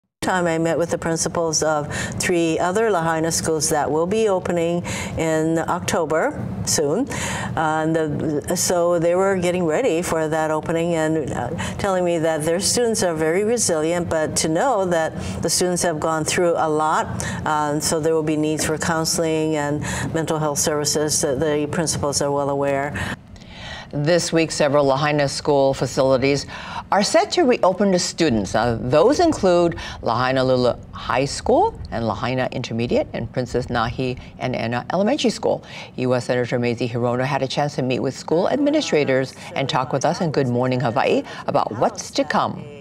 Lahaina School facilities ready to reopen for students this week
This week, multiple facilities at Lahaina School are scheduled to reopen for students, indicating a step towards resuming in-person learning.